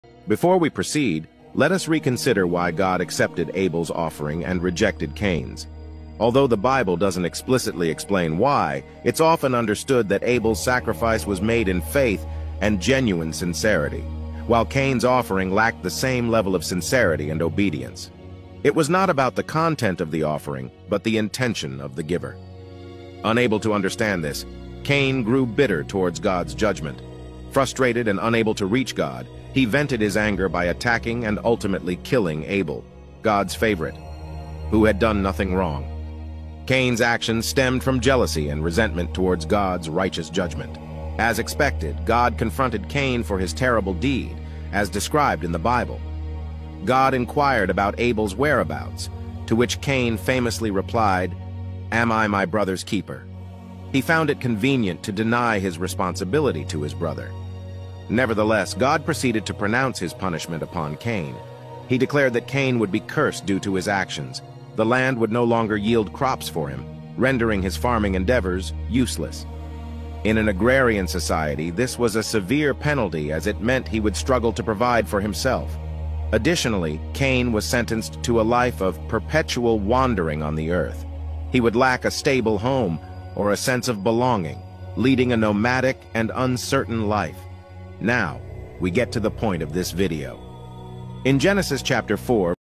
Cain and Abel -Bible Story Part 2
As they began to reap the benefits of their new occupations, they decided to give offerings to God to show their gratitude. For his offering, Abel brought God the “fatty portion” of his flock, which pleased Him. However, when Cain presented God with some of his harvest, He was not pleased.